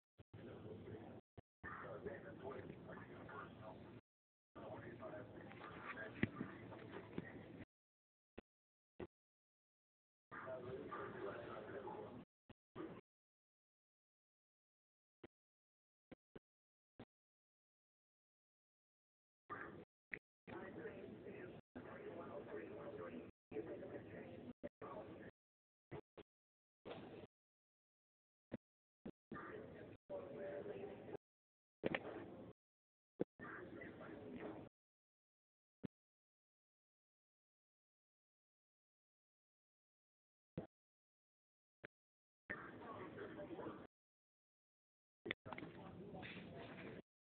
CTA Command Center - Public Safety Violations
Control Command Center Manager napping until my celphone video light starts to wake her up.